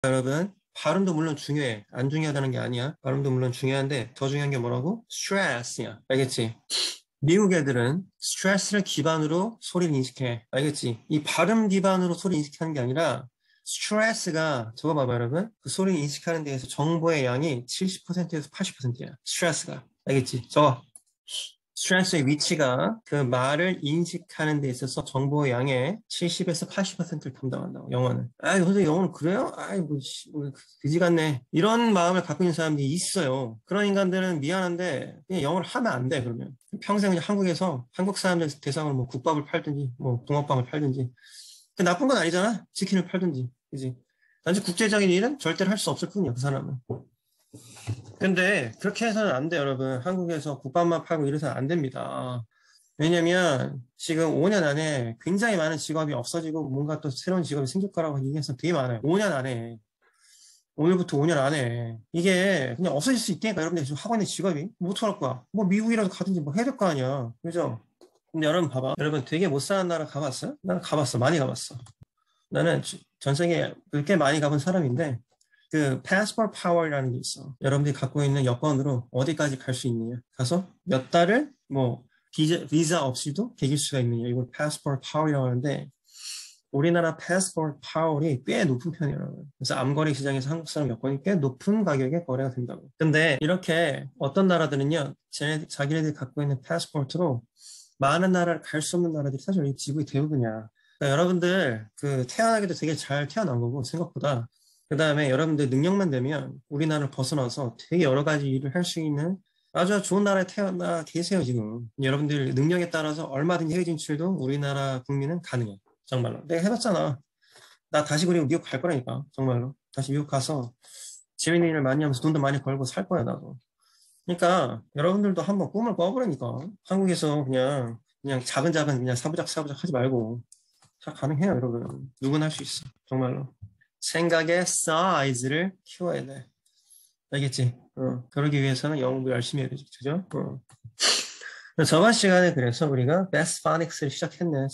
이렇게 세계 진출 하기에 좋은 나라에서 태어났는데 왜 섬나라 한국에 갖혀서 사는겨
제발 큰 세상으로 나가라구

저와함께  같이 영어 공부 하실 분들은
개인레슨 혹은 동영상 강의 구독 문의는 아래의 링크를 참고해주세요

레슨 후기는 아래 링크를 참고해주세요